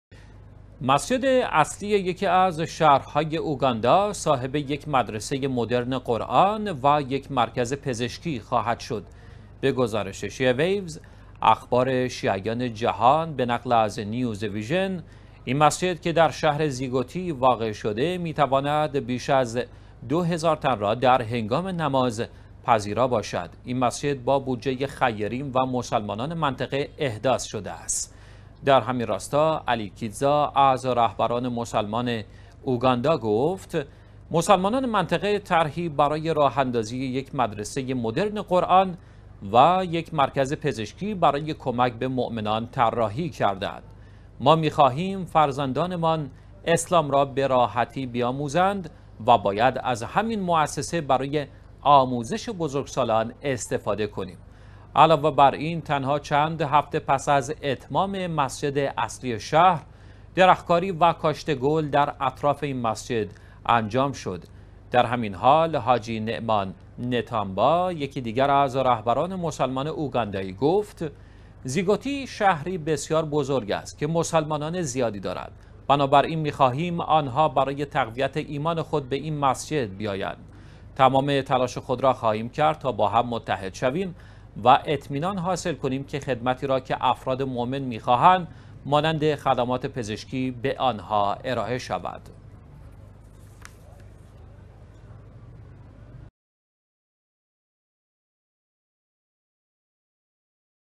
توسعه مسجد اوگاندا با مرکز مدرن قرآنی
مسجد اصلی یکی از شهرهای اوگاندا، صاحب یک مدرسه مدرن قرآن و یک مرکز پزشکی خواهد شد.
به گزارش شیعه ویوز (اخبار شیعیان جهان) به نقل از News Vision، این مسجد که در شهر زیگوتی واقع شده می‌ تواند بیش از ۲۰۰۰ تن را در هنگام نماز پذیرا باشد. این مسجد با بودجه خیرین و مسلمانان منطقه احداث شده است.
در همین راستا "علی کیتزا" از رهبران مسلمان اوگاندا گفت: مسلمانان منطقه طرحی برای راه‌ اندازی یک مدرسه مدرن قرآن و یک مرکز پزشکی برای کمک به مؤمنان طراحی کرده ‌اند. ما می‎ خواهیم فرزندانمان اسلام را به راحتی بیاموزند و باید از همین مؤسسه برای آموزش بزرگسالان استفاده كنیم. علاوه بر این تنها چند هفته پس از اتمام مسجد اصلی شهر، درختکاری و کاشت گل در اطراف این مسجد انجام شد.
در همین حال "حاجی نعمان نطامبا" یکی دیگر رهبران مسلمان اوگاندایی، گفت: زیگوتی شهری بسیار بزرگ است که مسلمانان زیادی دارد. بنابراین، می ‌خواهیم آنها برای تقویت ایمان خود به این مسجد بیایند. تمام تلاش خود را خواهیم کرد تا با هم متحد شویم و اطمینان حاصل کنیم که خدمتی را که افراد مؤمن می ‌خواهند مانند خدمات پزشکی به آنها ارائه شود.